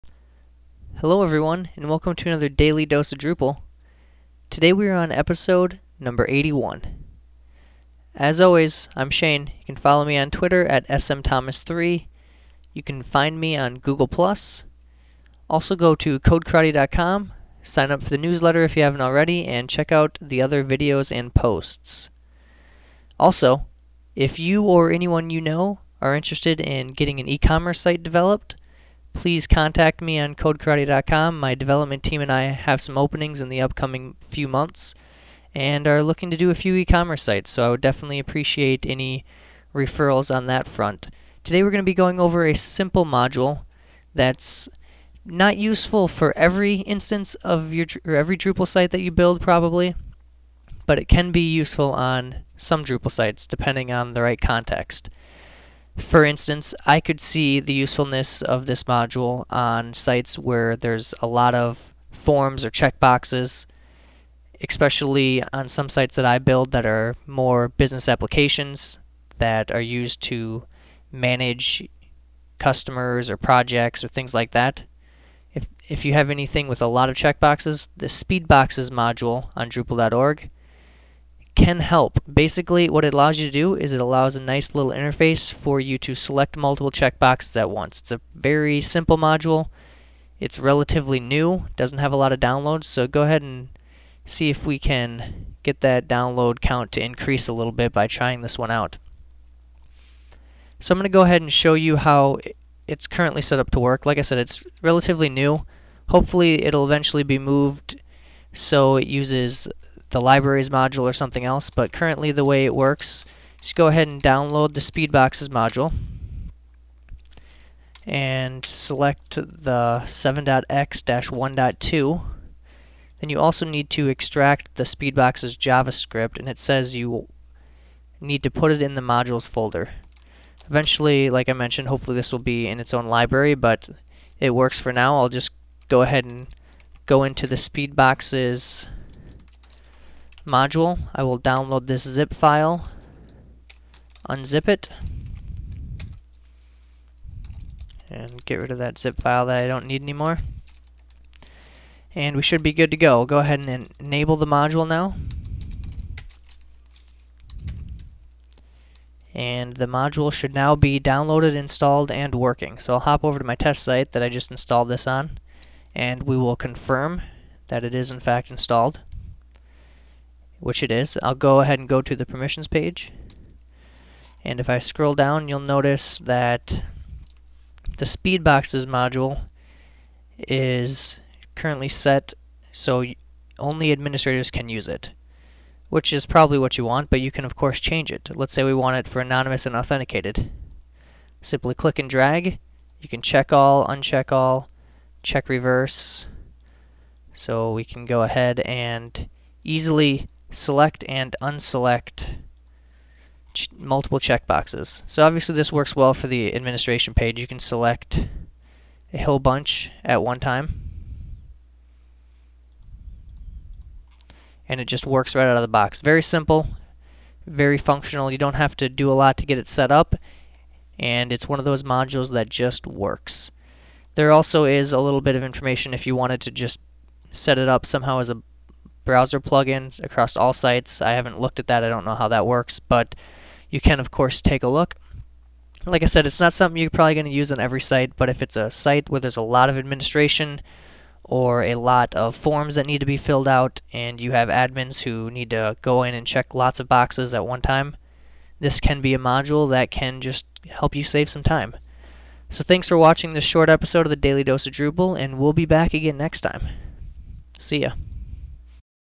Drupal 7 Speedboxes module - Daily Dose of Drupal episode 81
The Drupal 7 Speedboxes modules makes the checking and unchecking of checkboxes on your Drupal 7 site simple. The module is pretty easy to get installed and provides a draggable interface for multiple checkbox selection/deselection.

In this episode you will learn:

- How to download and install the Drupal 7 Speedboxes module
- How to use the Drupal 7 Speedboxes module